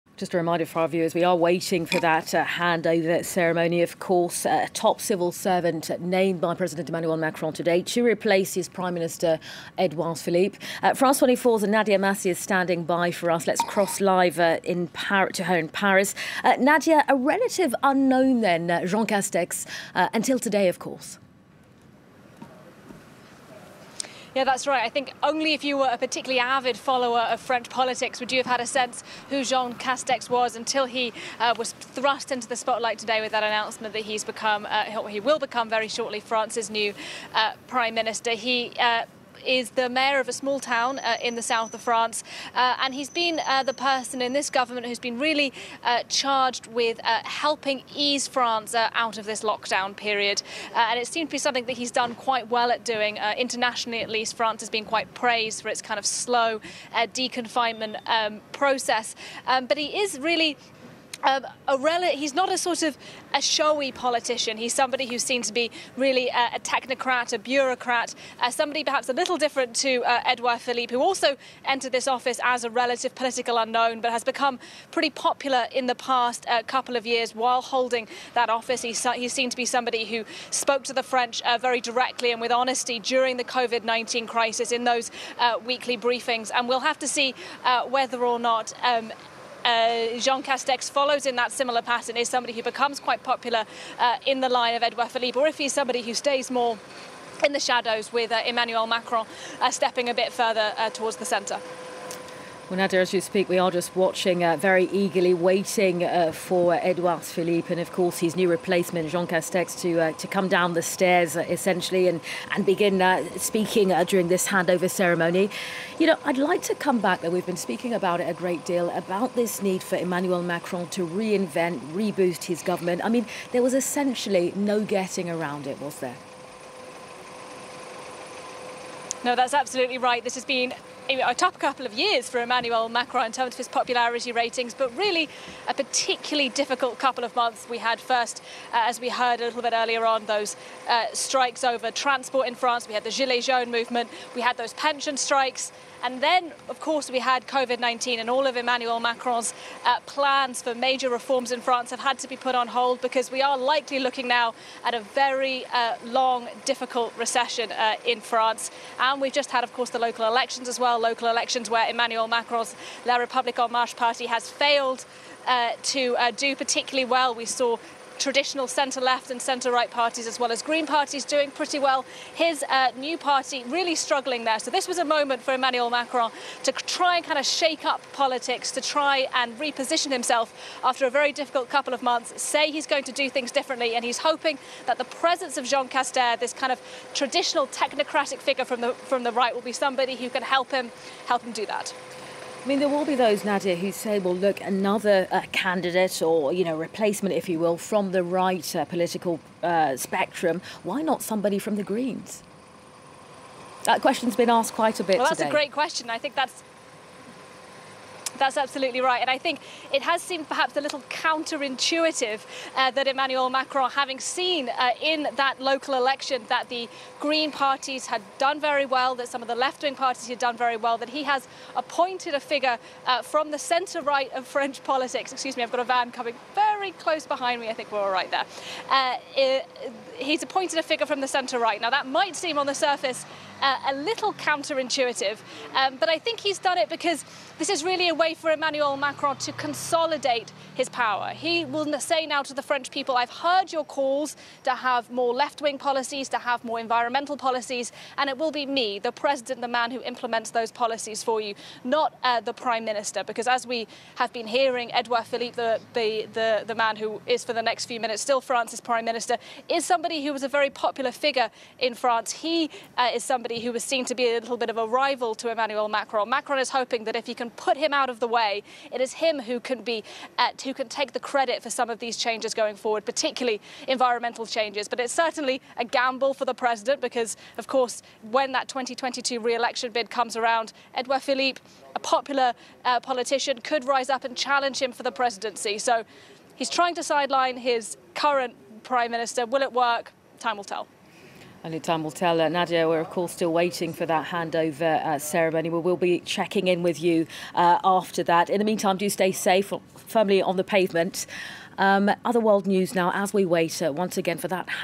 Who is Jean Castex, the rather unknown' new French Prime Minister
French President Emmanuel Macron has named Jean Castex the new Prime minister after the resignation of the Édouard Philippe cabinet. Rather unknown to the public, Castex came back to the public light early April when appointed to head the government strategy for the end of coronavirus lockdown measures. “This reshuffle makes total sense, Macron wants to start a new chapter for the 2022 elections”, Ariane Bogain, a senior lecturer at the Northumbria University in Newcastle, told FRANCE 24.